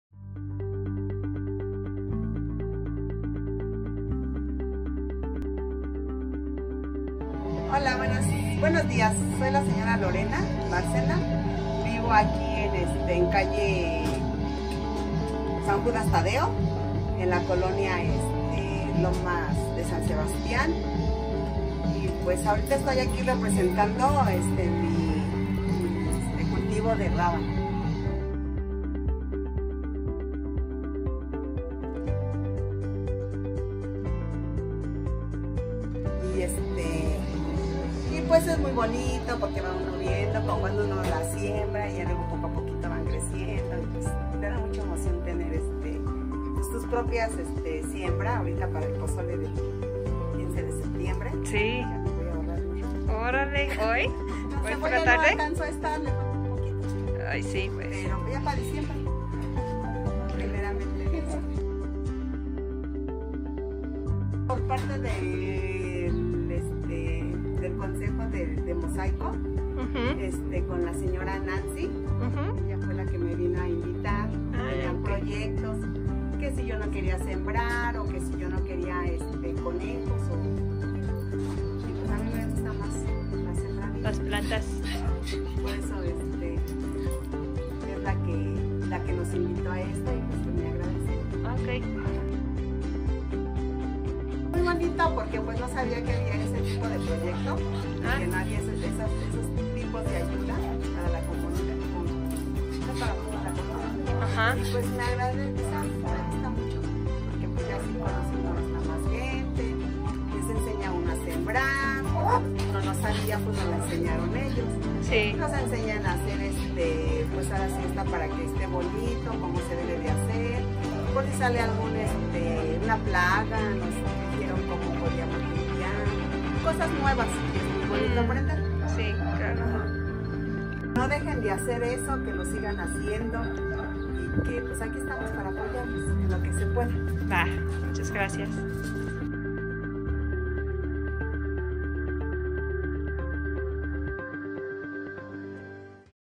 Set the Table: Lorena's Story
Listen to Lorena as she shares her experience participating in Urban Mosaic's Food Security Project. This project is one of the many ways the UM team is responding to the crisis caused by COVID-19. What started as a public health crisis has quickly become a growing food crisis.